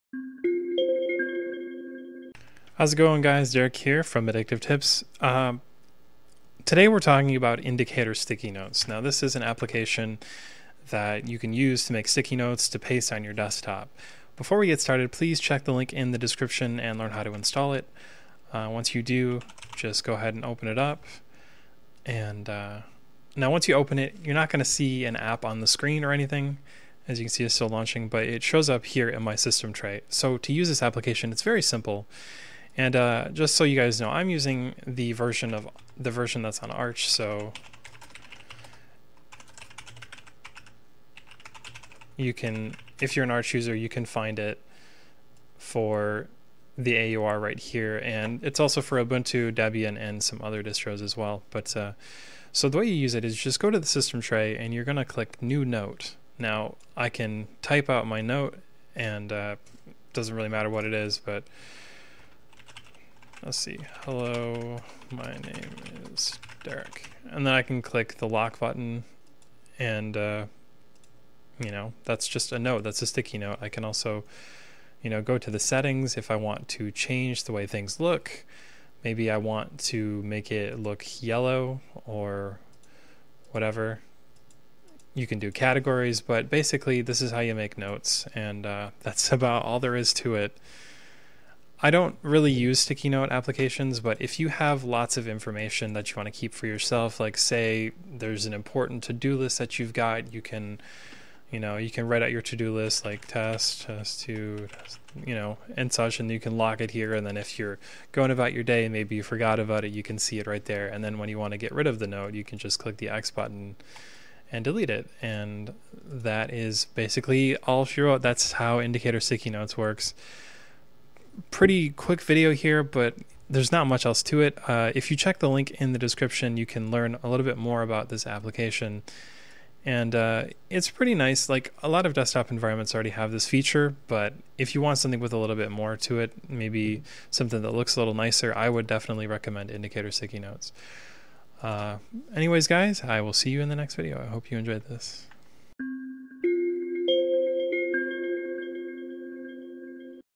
How To Save Sticky Notes On Linux With Indicator Stickynotes
Indicator Stickynotes is a note-taking application for saving sticky notes on Linux. It allows users to create graphical, “post-it note” style notes to paste on the Linux desktop.